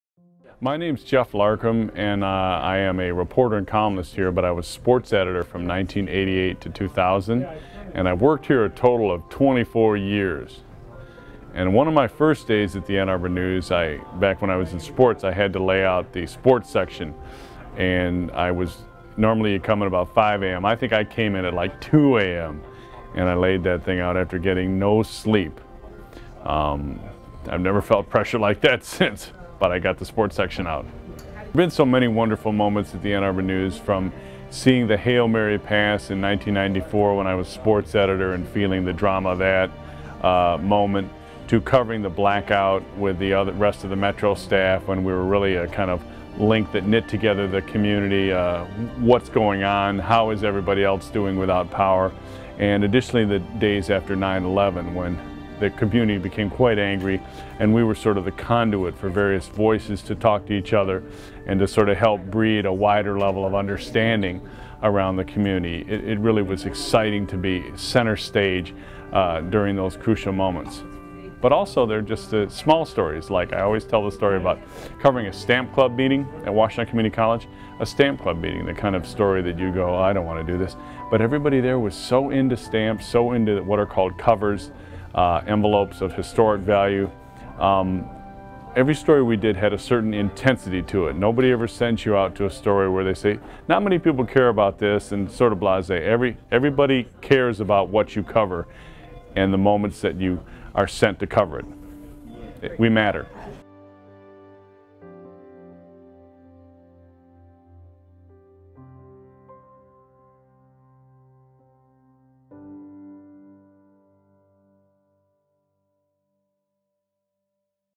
Geoff Larcom: Ann Arbor News Memories
Geoff Larcom enjoyed a long career at The News, which included stints as assistant sports editor (1982-85), sports editor (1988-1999) and as a metro desk higher education reporter and local columnist (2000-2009). He loved working for his hometown paper, a place where fantastic stories were literally within walking distance and where it was so easy to connect with readers and the many elements of a fascinating town.